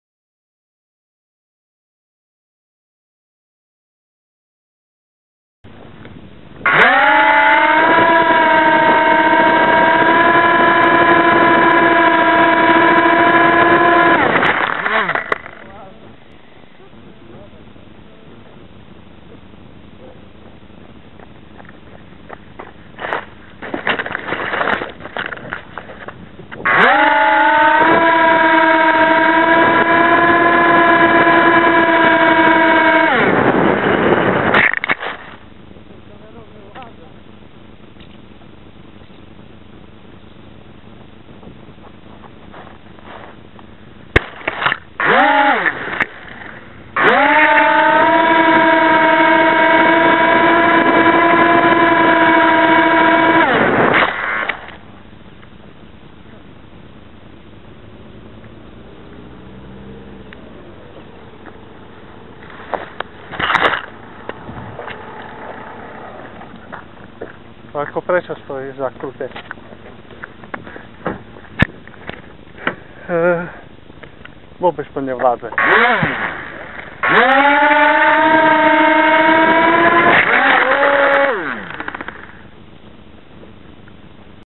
môj prvý let s kamerou
mala síce lne 20g, ale B17 ani tak nevládala lietať